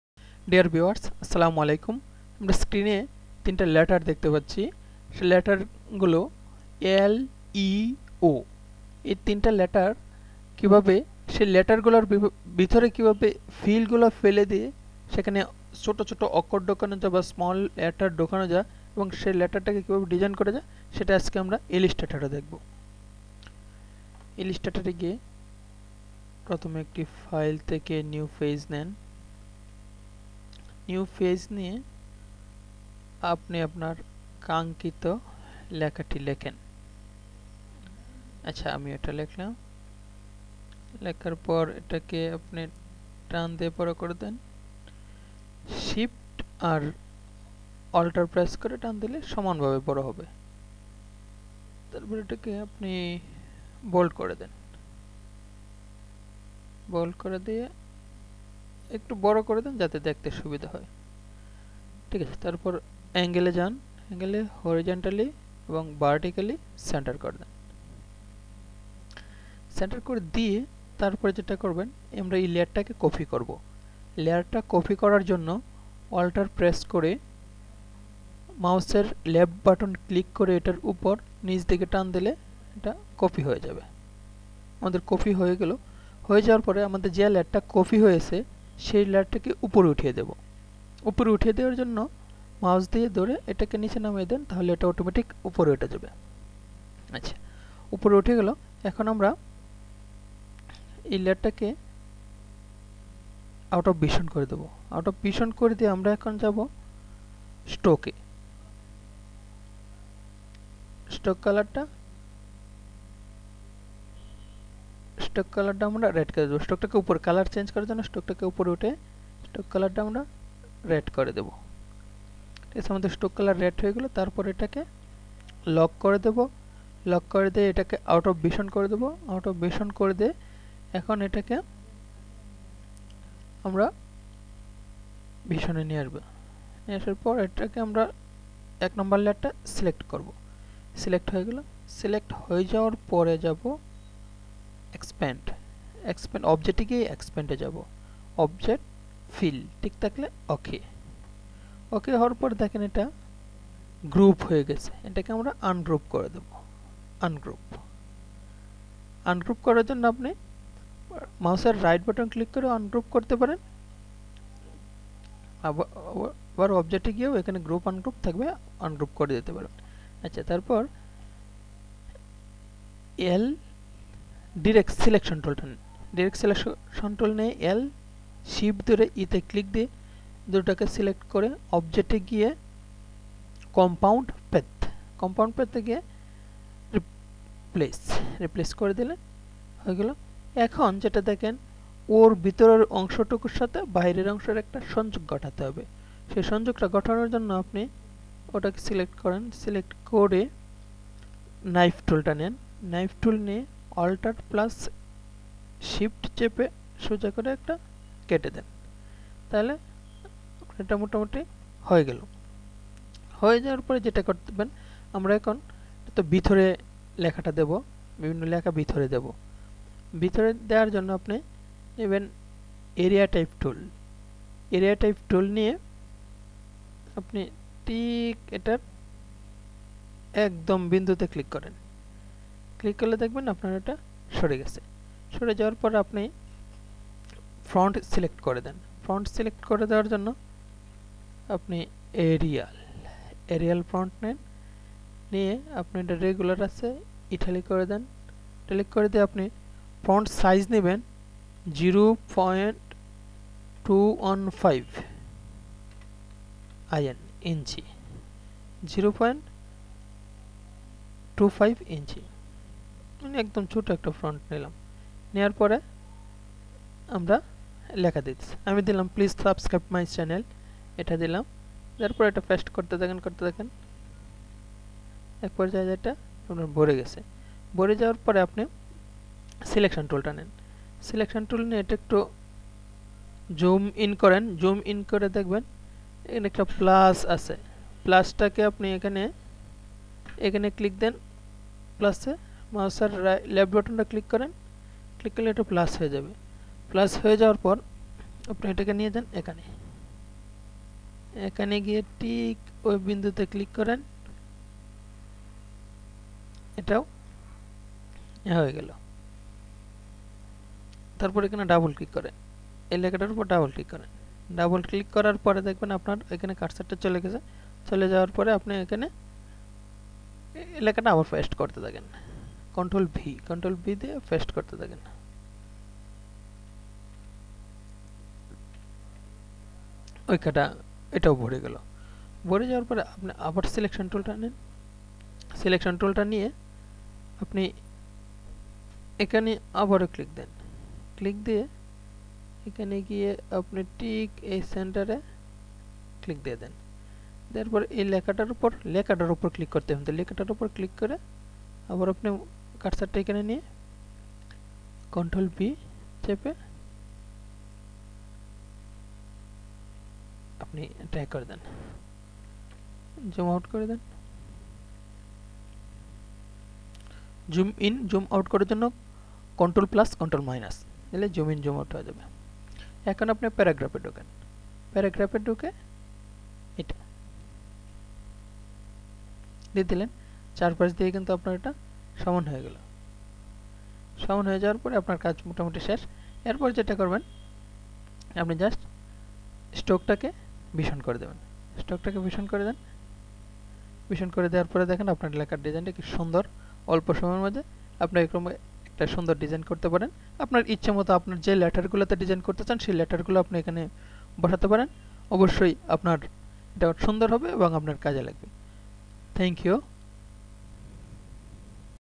How to Fill Letter Shapes with Link Threaded Text in Adobe Illustrator
Fill Letter Shapes with Link Threaded Text in Adobe Illustrator